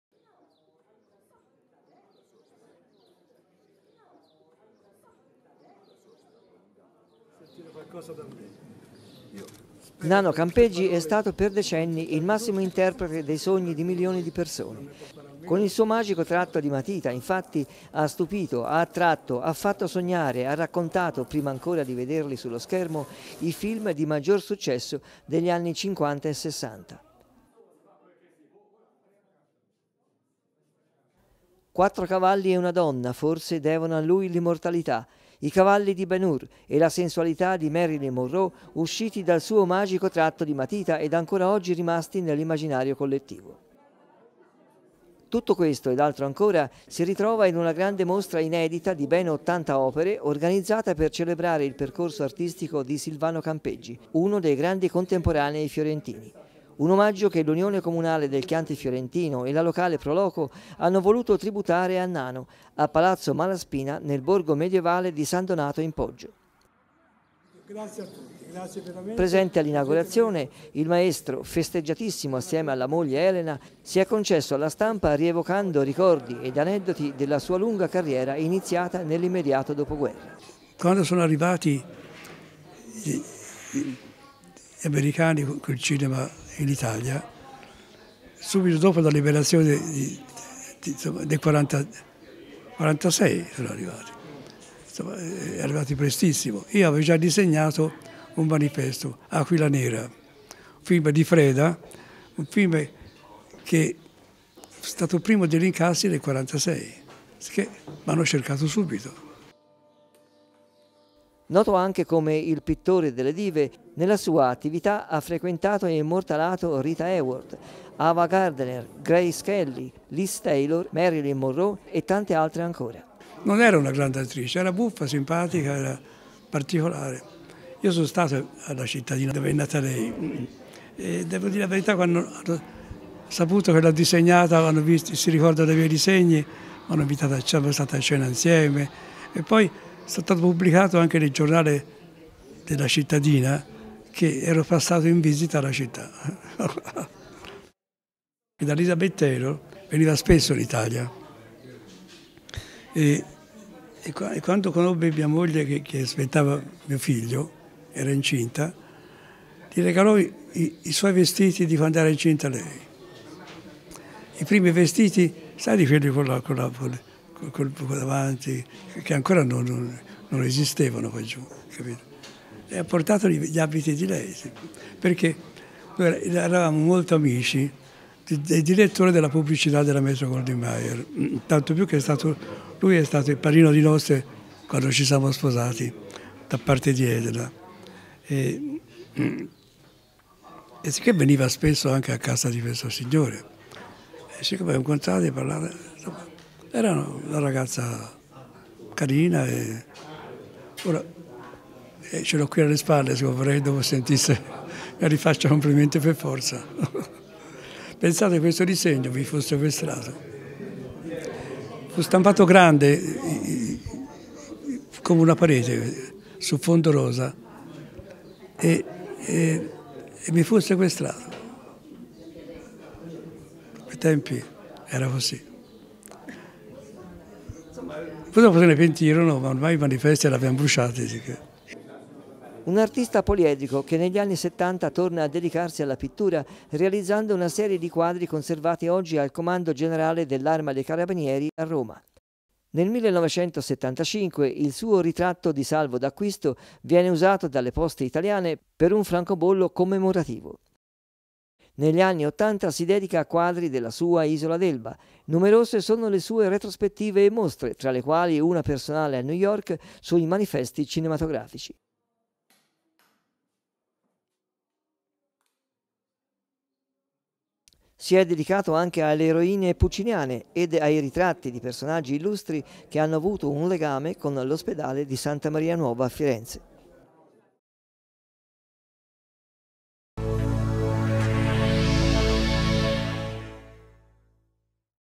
Nano CAMPEGGI, il film di una vita artistica
Ottanta opere di Silvano Campeggi a Palazzo Malaspina nel borgo medioevale di San Donato in Poggio.